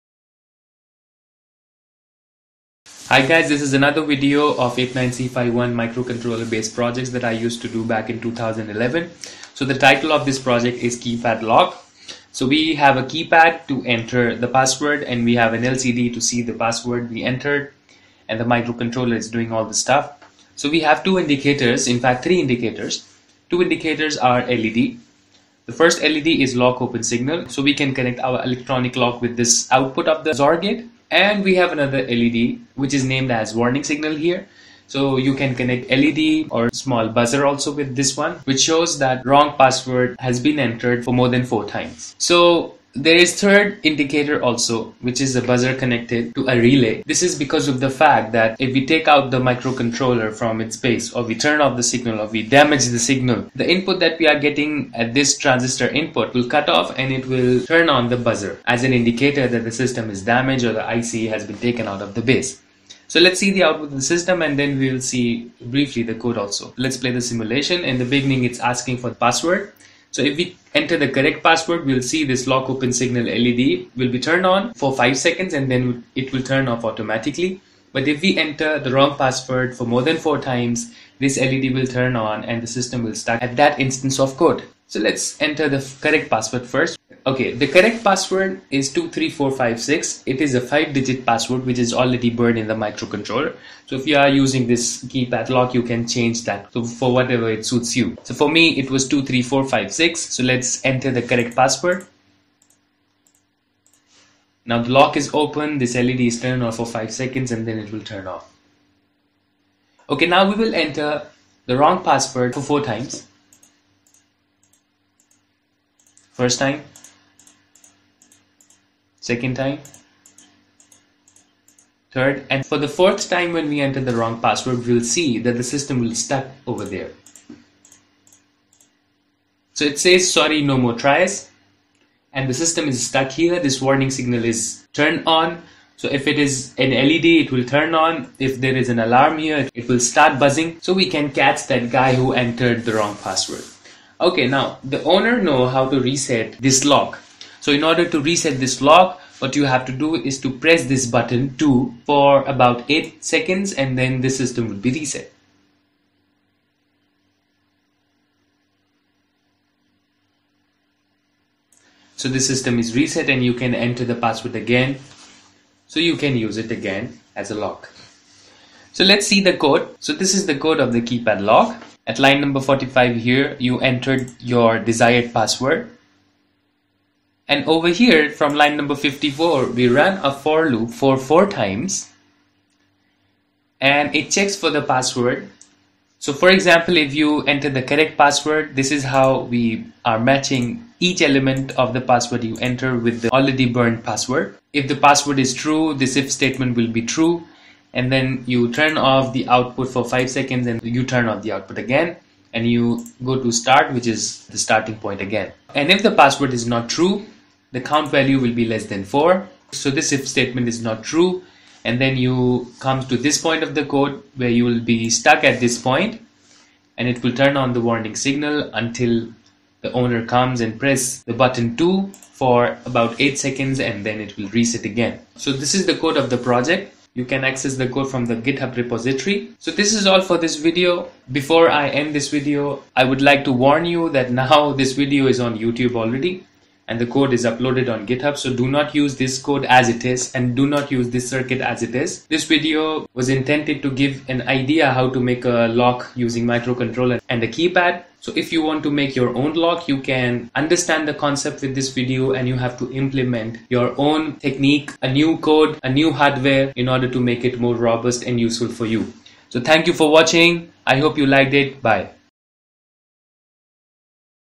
8051 Based Electronic Keypad Lock
This project is an 8051 based electronic keypad lock. A 5-digit password is hard-coded in the micrcontroller. The lock asks the user to enter the password with 4 trials available. If the password is correct, output is given in 2 hardware bits of microcontroller which are connected externally to a XOR gate to prevent switching of the gate/lock when both the inputs are HIGH or LOW. If password is entered wrong four times, another signal (warning signal) is activated in a similar fashion and the system is locked at this point in the code until the owner resets it by pressing "2" for about eight seconds.

For greater protection, a buzzer is connected to 1-bit of microcontroller which is always HIGH. If the controller is taken out of the base or the IC is damaged or the power supply is disconnected the relay will change its state and turn on the alarm/buzzer.

The Proteus file and the code can be accessed from the git repository which is linked as follows: